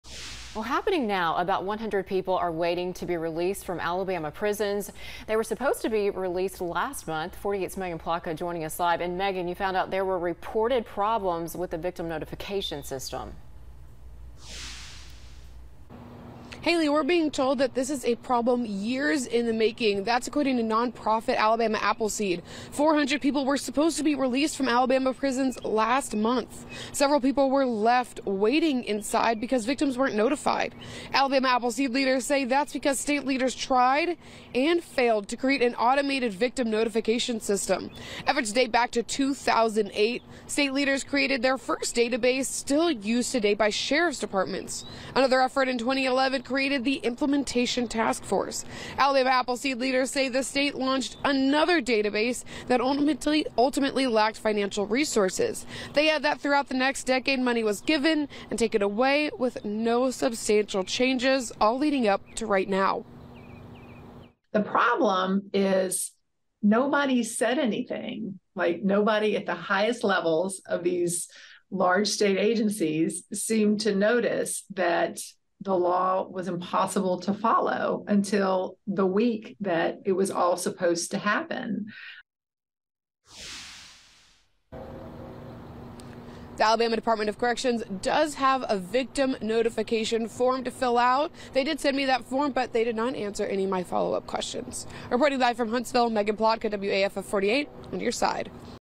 Advocates in Alabama push for streamlining of victim notification system
WAFF's Megan Plotka reporting